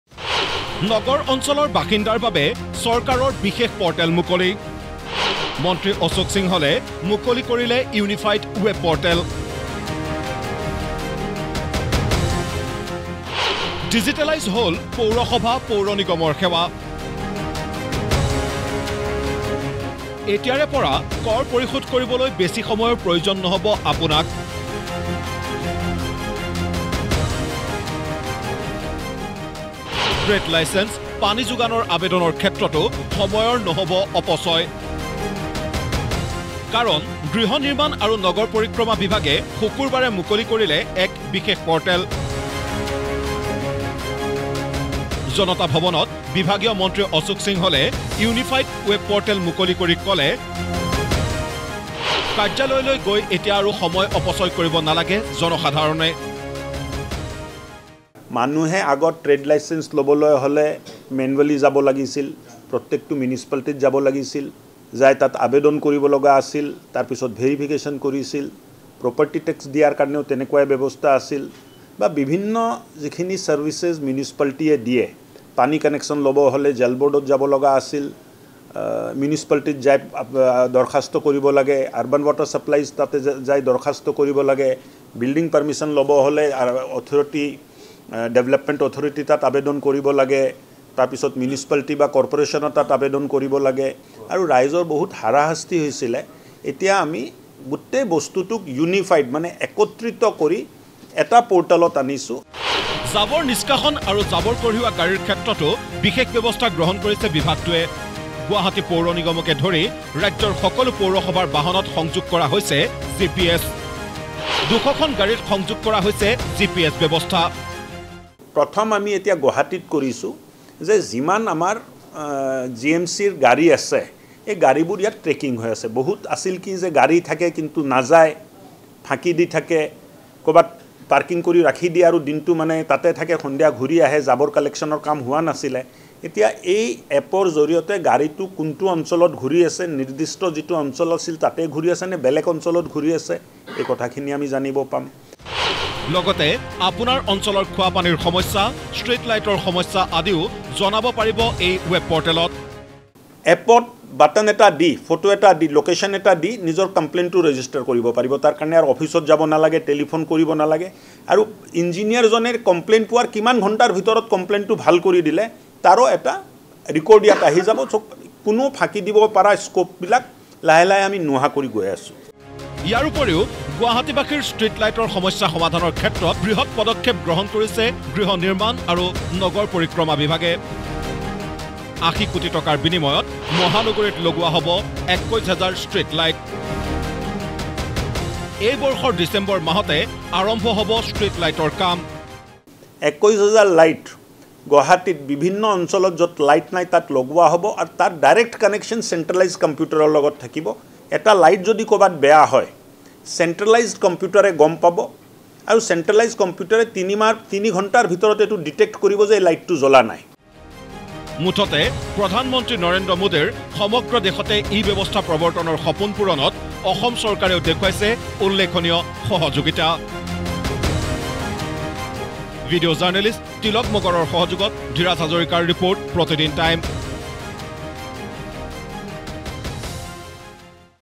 ৰাজ্যৰ নগৰ অঞ্চলৰ বাসিন্দাৰ বাবে চৰকাৰে মুকলি কৰিলে এক বিশেষ ৱেব প’ৰ্টেল
ৰাজ্যৰ নগৰ অঞ্চলৰ বাসিন্দাৰ বাবে চৰকাৰে মুকলি কৰিলে এক বিশেষ ৱেব প’ৰ্টেল। শুকুৰবাৰে গৃহ নিৰ্মাণ আৰু নগৰ পৰিক্রমা বিভাগৰ মন্ত্রী অশোক সিংহালে মুকলি কৰিলে এই ৱেব প’ৰ্টেল। জনতা ভৱনত মুকলি কৰা এই প’ৰ্টেলৰ জৰিয়তে কি সুবিধা লাভ কৰিব জনসাধাৰণে? চাওক আমাৰ এক ৰিপ’ৰ্ট।